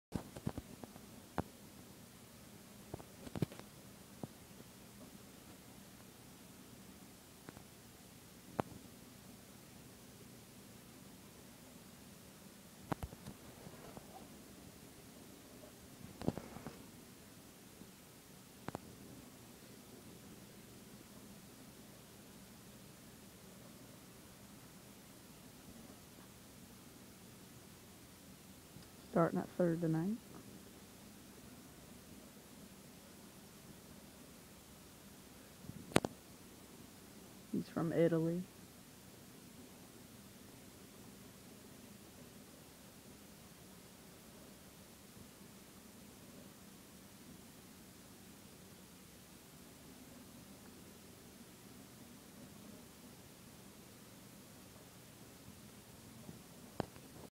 Alex liddy major league debut at bat
Seattle mariners rookies first at bat